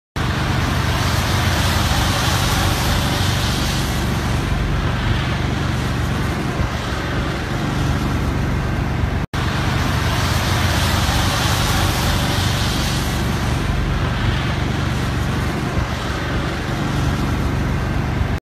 Truck drivers shovel snow off roofs after being pulled over by state police
New Hampshire State Police troopers pulled over truck drivers Monday morning so they could shovel snow off their roofs to be in compliance with Jessica's Law.